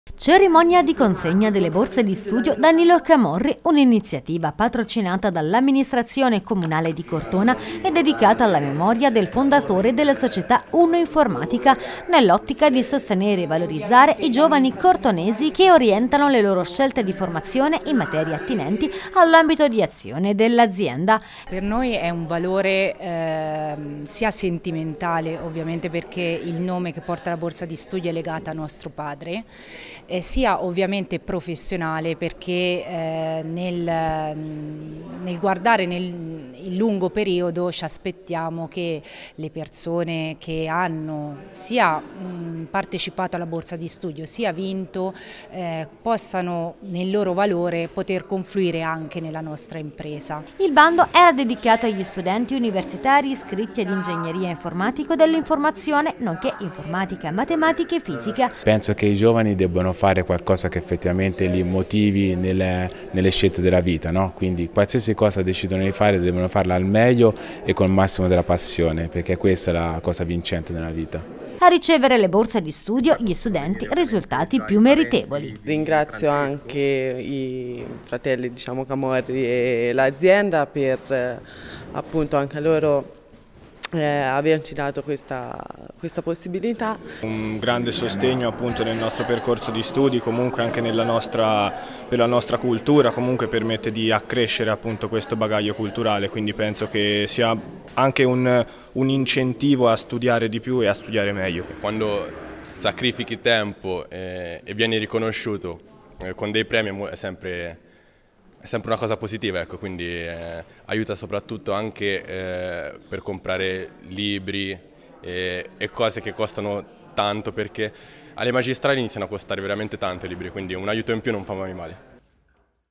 Consegnate le borse di studio "Danilo Camorri": tre studenti premiati
Cerimonia di consegna delle borse di studio “Danilo Camorri”, un'iniziativa patrocinata dall'amministrazione comunale di Cortona e dedicata alla memoria del fondatore della società Uno Informatica, nell’ottica di sostenere e valorizzare i giovani cortonesi che orientano le loro scelte di formazione in materie attinenti all’ambito di azione dell’azienda.
A ricevere le borse di studio sono stati gli studenti risultati più meritevoli.